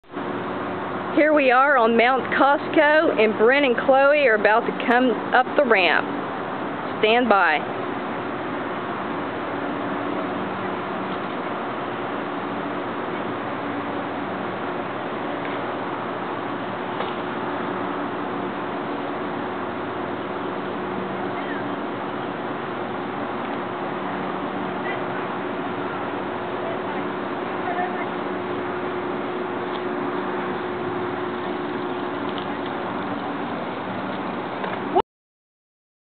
Chloe and I practice our climbing skills on a local urban mountain.
Our 1 1/2 year old westie LOVES to go bike riding!